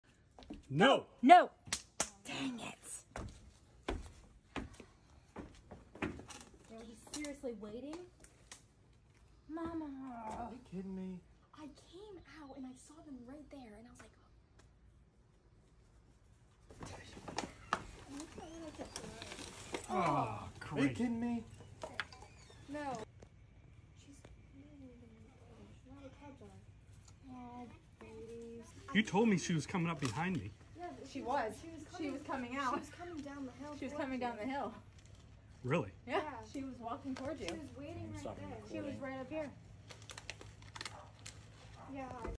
BEAR Family Steals Dinner
Stay Bear Safe

While on vacation this year, my family and I rented a cabin in a neighborhood that is known to have bears. One day upon arriving back to our cabin after walking our dog, we found a mother bear with three cubs sniffing around our bear-proof garbage enclosure. We waited at a safe distance until the bear family moved along and into the woods behind the house.

A few days later, as I was placing a bag of garbage in the enclosure, my daughter just happened to walk out onto the deck and shouted to me, “Dad, there’s a bear coming right behind you”. The carabiner for the garbage enclosure was rusted and difficult to fit through the holes to secure the trash and not knowing exactly how close she was, I was forced to move towards the deck steps into a safer position. I really wish that I had turned around to see exactly how far away she was. After the incident was over we approximated her to be 20 feet away. We’ll never know if I would have had enough time to lock the cans up.

Because of the previous incident, I was sure to look all around the cabin just to be sure that I wasn’t carrying a garbage bag right to a waiting, hungry bear family. I’m fairly certain that the sow was somewhere in the woods behind the cabin and both smelled the garbage and heard the sound of the garbage enclosure opening which caused her to bring her three cubs in for an easy meal.

Unfortunately, this is not the first time these bears have fed from a garbage can. The mother bear was likely taught this behavior as a cub and now she is teaching her cubs. Situations like mine can happen…and I’m someone who has experienced too many bear encounters to count. If you vacation in bear country, be aware of your surroundings and do your absolute best to secure your trash cans. Wild bears should eat wild food. Once bears have a taste for human food, issues like this bear approaching close behind me become more frequent and the potential for a negative conflict increases.